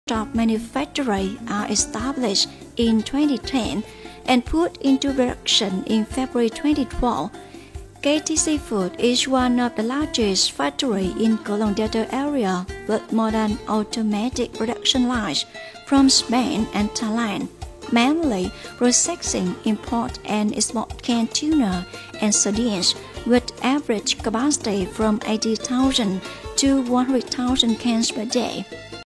Canned Sardines factory
The process of fishing, cleaning, packaging, processing and production of canned food.